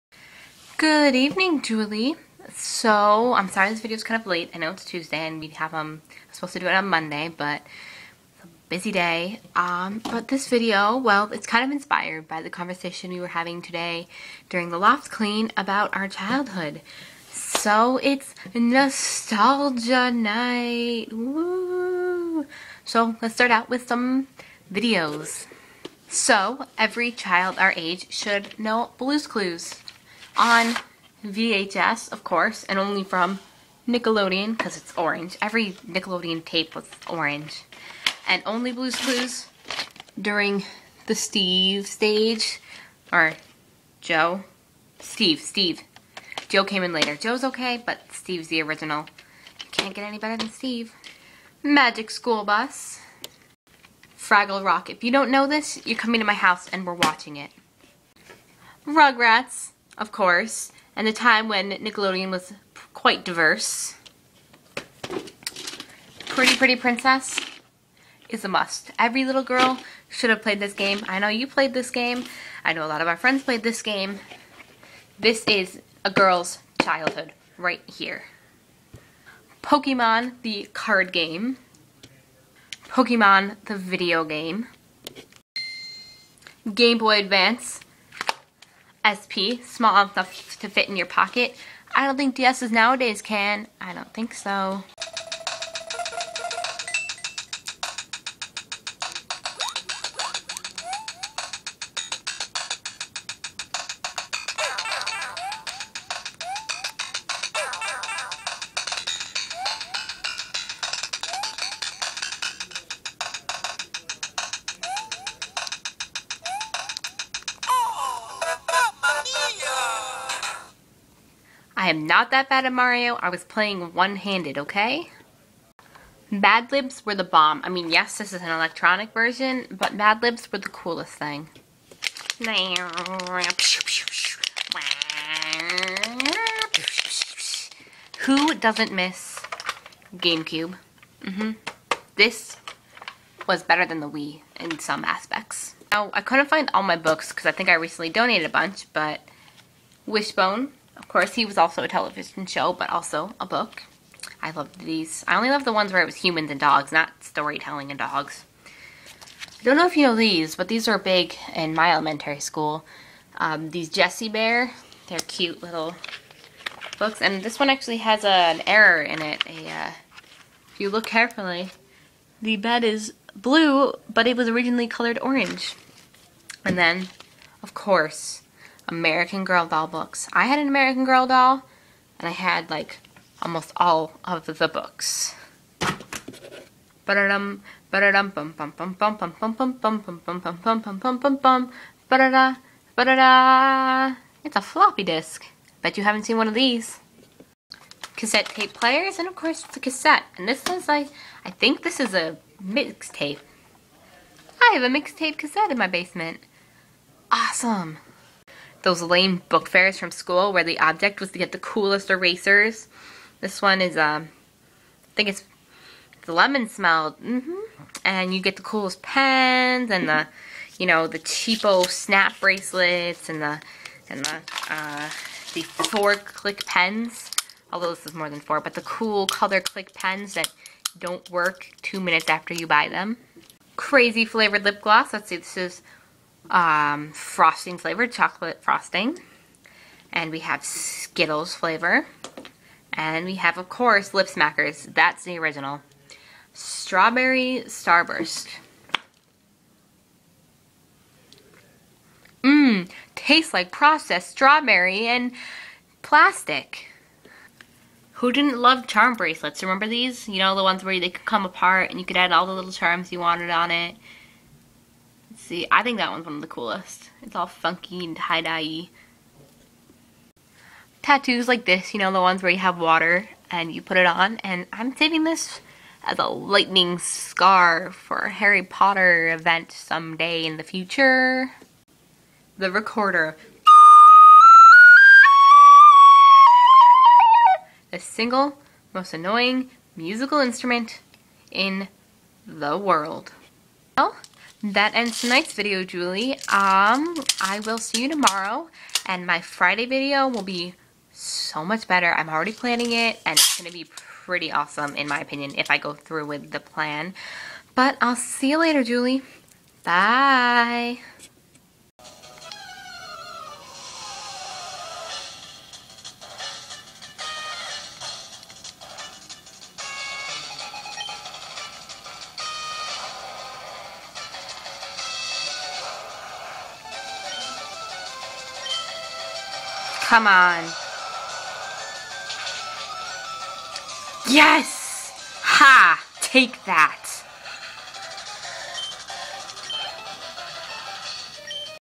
Nostalgia Night
90s/ childhood items and memories. Sorry its a little long